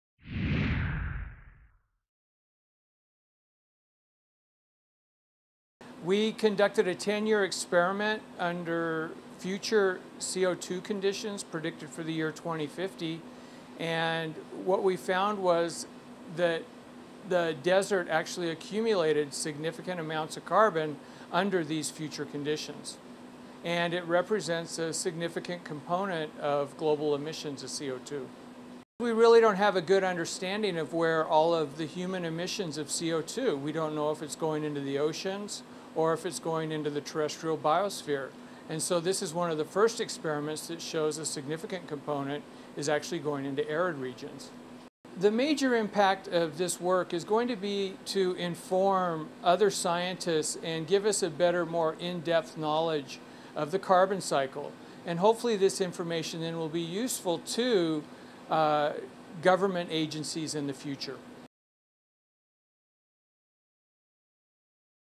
Research: Arid areas absorb unexpected amounts of carbon
PULLMAN, Wash. - Researchers led by a Washington State University biologist have found that arid areas, among the biggest ecosystems on the planet, take up an unexpectedly large amount of carbon as levels of carbon dioxide increase in the atmosphere. The findings give scientists a better handle on the earth's carbon budget - how much carbon remains in the atmosphere as CO2, contributing to global warming, and how much gets stored in the land or ocean in other carbon-containing forms.